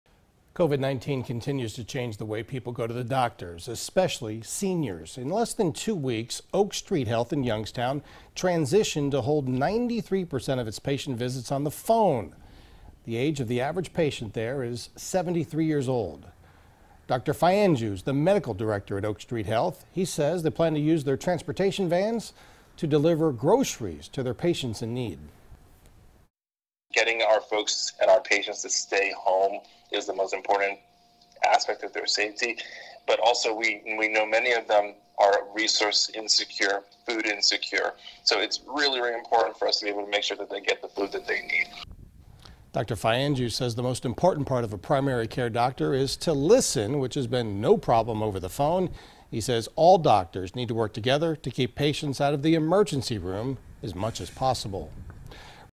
Oak Street Health makes changes to support senior patients amid COVID-19 outbreak
In less than two weeks, Oak Street Health in Youngstown transitioned to hold 93% of its patient visits over the phone